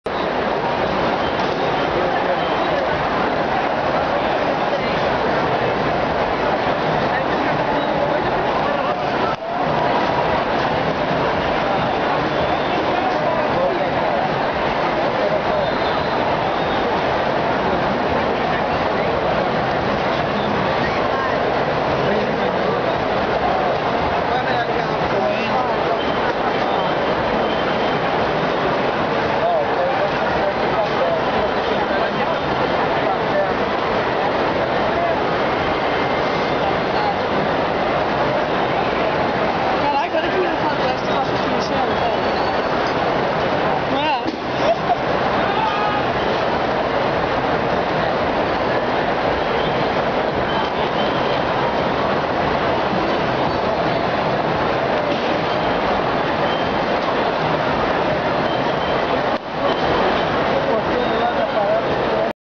caracol de pessoas saindo do show do Paul McCartney
pessoas saindo do show, um caracol humano